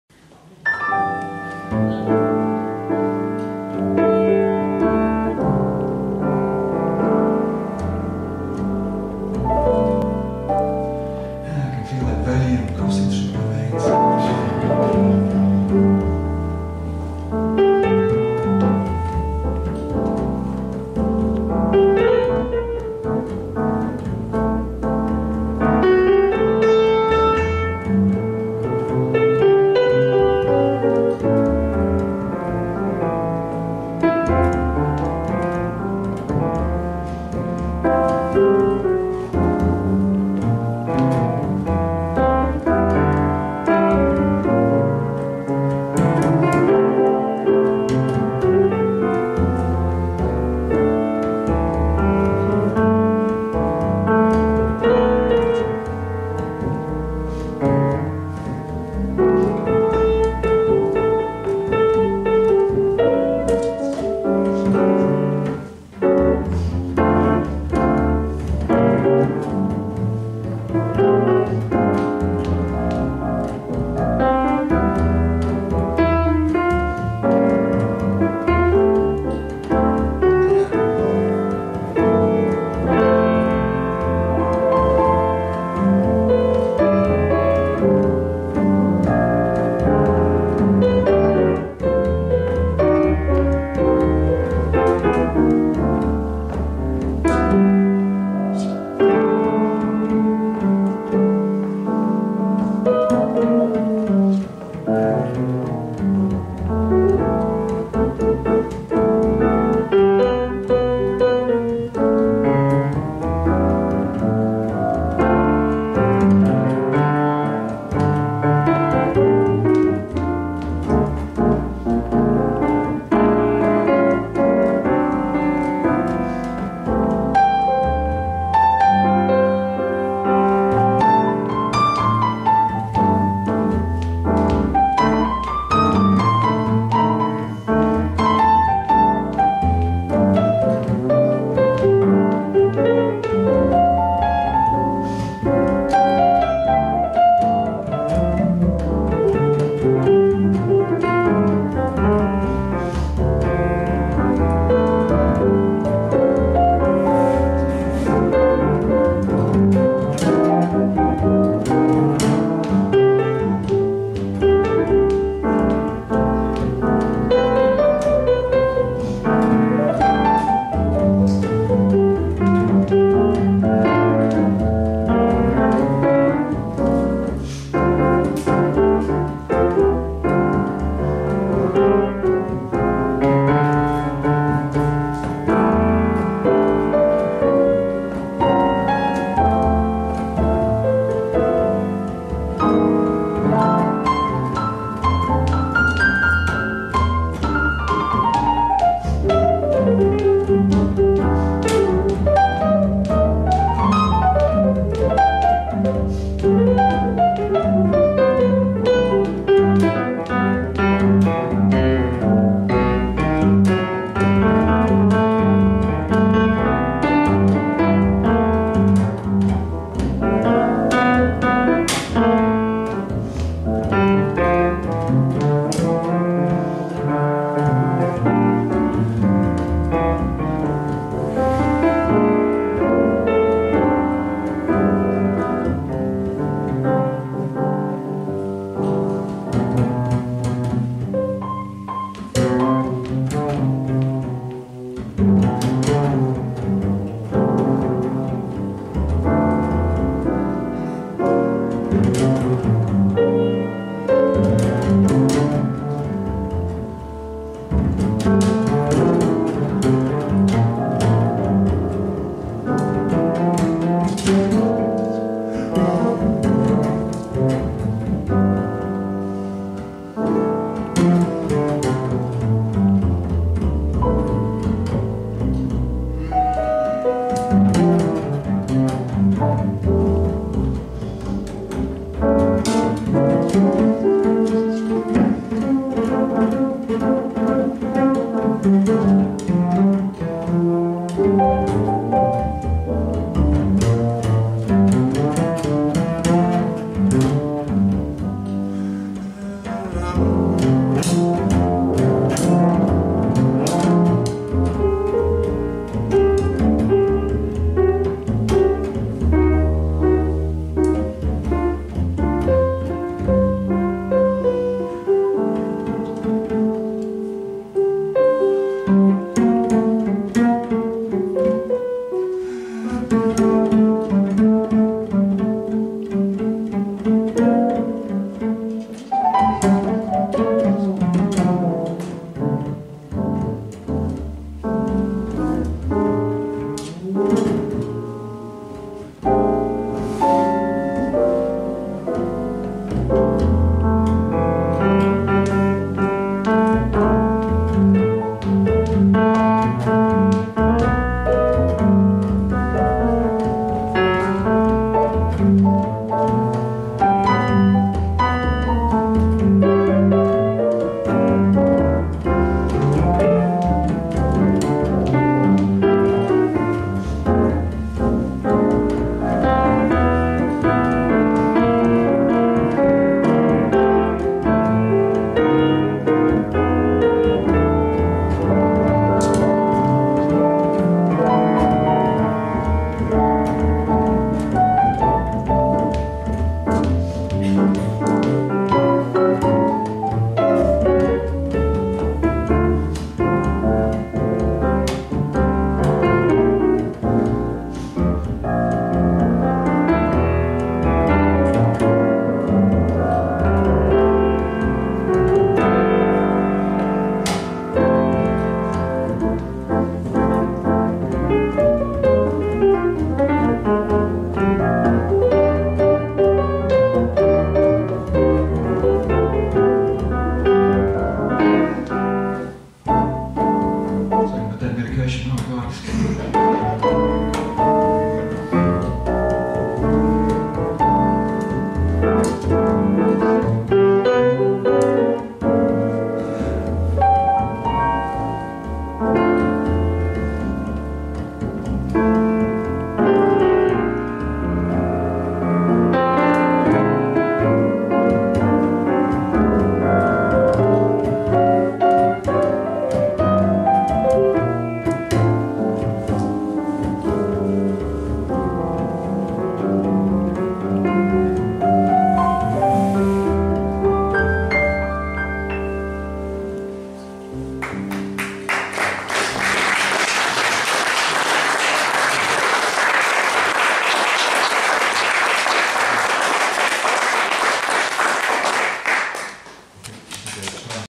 John Bevin & Mike Kime 2017 NPC B
Warming Up
John Bevin - Piano
Mike Kime - Double Bass
Evening of Classical Jazz